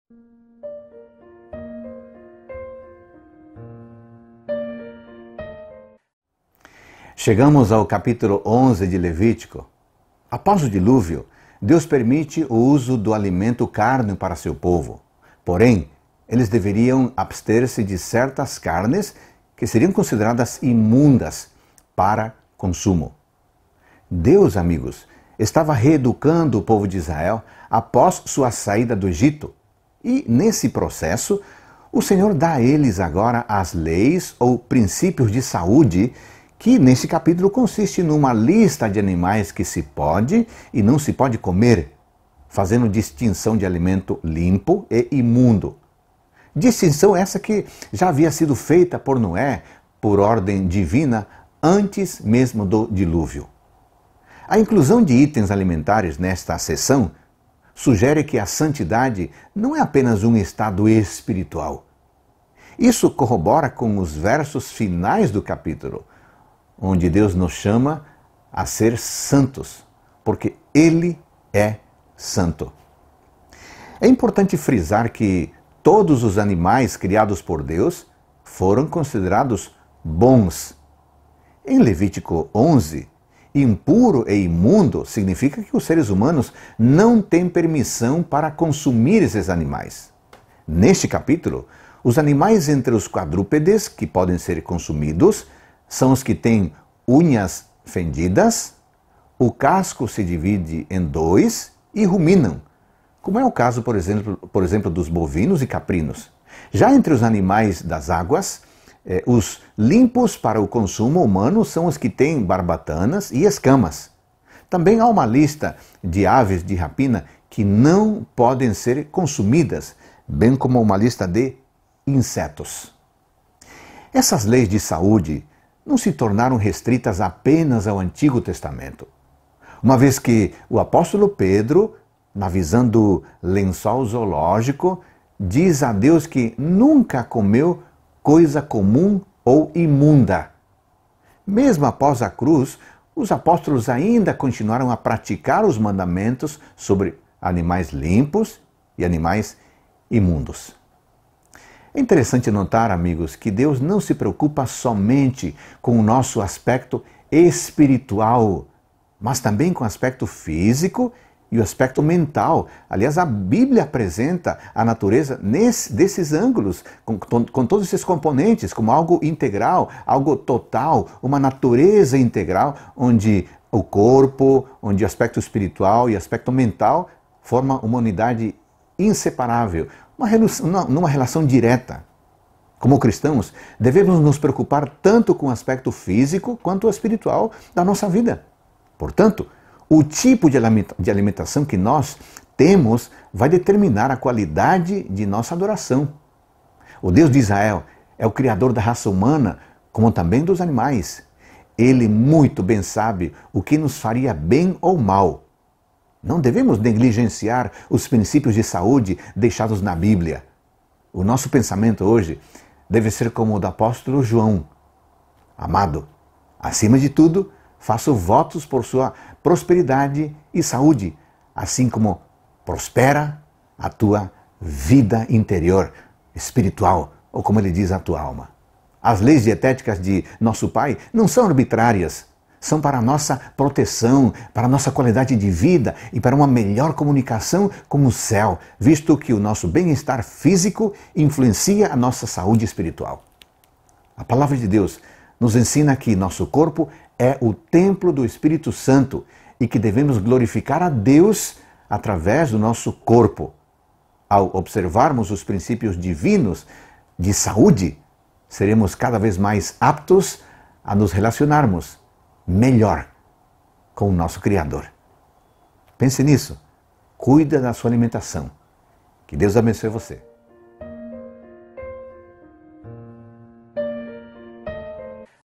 Levítico 11 - Reavivados por Sua Palavra
Após o diluvio, Deus permite o uso do alimento cárneo para Seu povo. Porém, eles deveriam abster-se de certas carnes que seriam consideradas imundas para consumo.